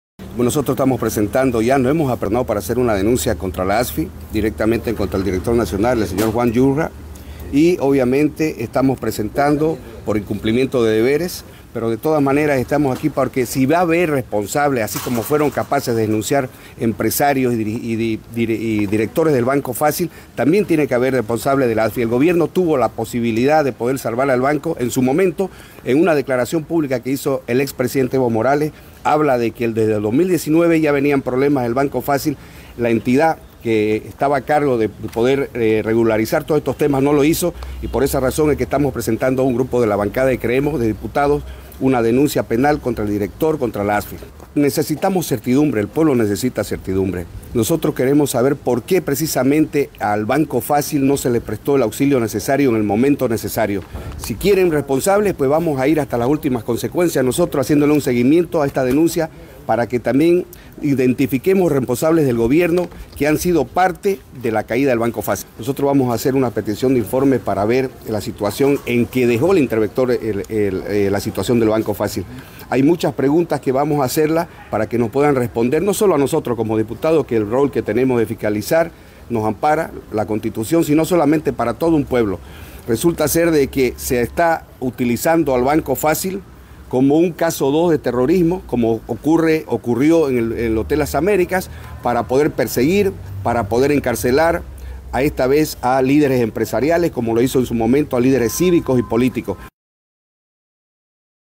Diputado Saucedo de Creemos formalizó demanda penal contra Director Ejecutivo ASFI por no
Diputado Saucedo de Creemos formalizó demanda penal contra Director Ejecutivo ASFI por no salvar en su momento al ex Banco Fassil

Jorge Saucedo Diputado de Creemos, presentó hoy ante la fiscalia una denuncia penal contra el Director Ejecutivo de la ASFI Reynaldo Yujra, por incumplimiento de deberes y otros delitos con relación al Ex Banco Fassil, porque hay las dudas sobre que hicieron  desde qué se enteraron de la situación de la entidad financiera y qué hicieron o no? para ayudarla a salir del problema. Saucedo, aseguró qué llegarán hasta las últimas consecuencias, porque no puede ser posible qué el caso Banco Fassil se esté utilizando comp un caso terrorismo para perseguir y encarcelar empresarios cruceños.